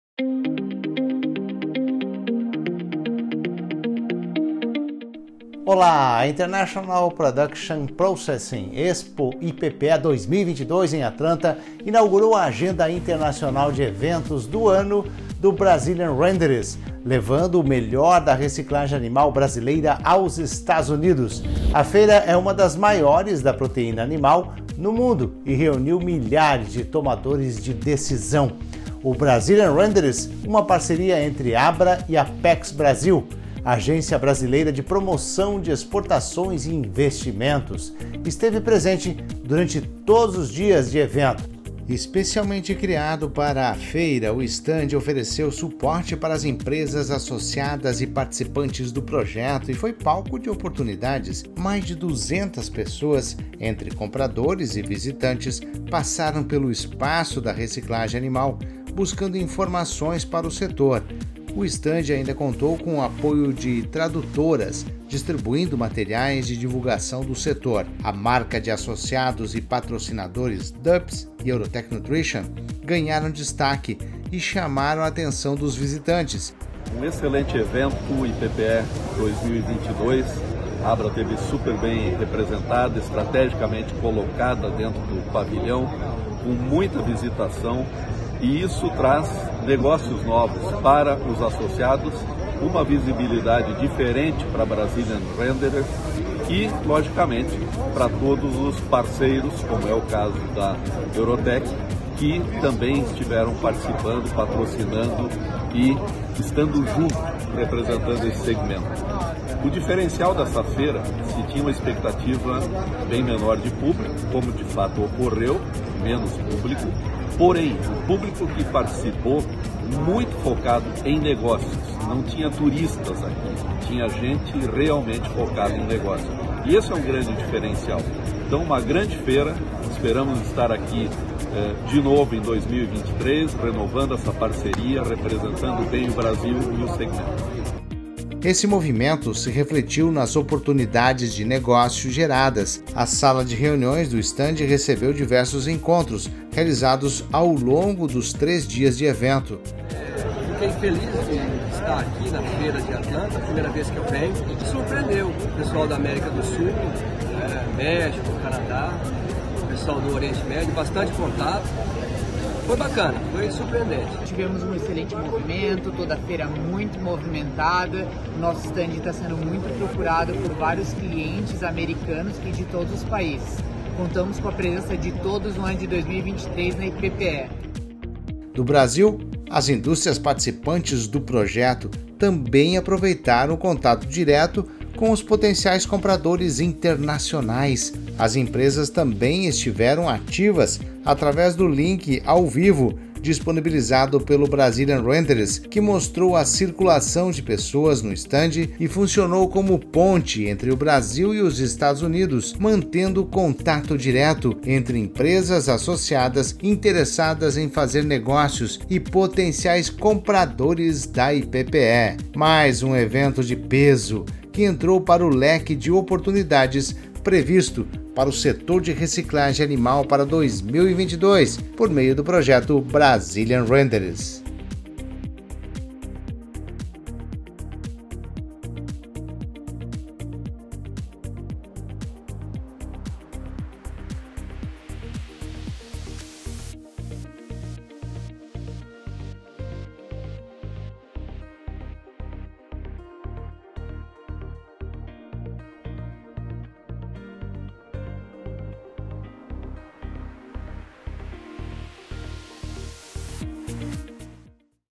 Brazilian Renderers • IPPE 2022
A International Production Processing Expo (IPPE 2022), em Atlanta, inaugurou a agenda internacional de eventos do ano do Brazilian Renderers, levando o melhor da reciclagem animal brasileira aos Estados Unidos. A Feira é uma das maiores da proteína animal no mundo e reuniu milhares tomadores de decisão de todo globo.

O Brazilian Renderers, uma parceria entre ABRA e ApexBrasil (Agência Brasileira de Promoção de Exportações e Investimentos), esteve presente durante todos os dias de evento, de 25 a 27 de janeiro.